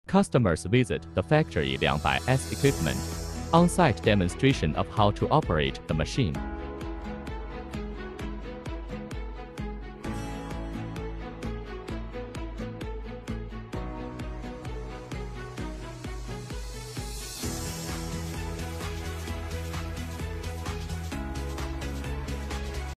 【Customer factory inspection】compost
French customers visited our factory and used equipment to detect operating noise. Watch the equipment operation on site.